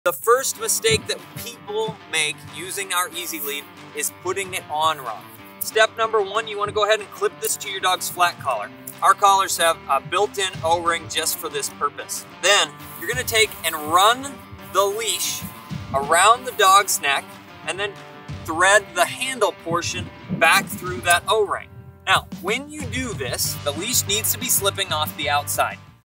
How to Use Standing Stone’s Easy Lead
If your Easy Lead isn’t working... it might not be on right!

5919 W Pleasant Valley Rd

Produced by: Red 11 Media - red11media.com